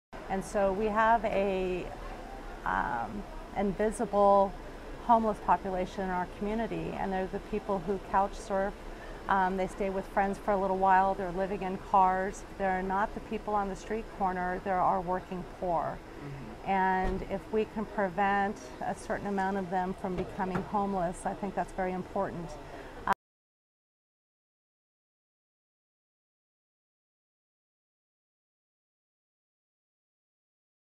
Bellingham Ward 5 candidate Lisa Anderson: Homeless population needs help
Lisa Anderson is a 2019 general election candidate for Bellingham, Wash., City Council Ward 5. She says there is an invisible homeless population that needs assistance.

Video by Warren Sterling | The Bellingham Herald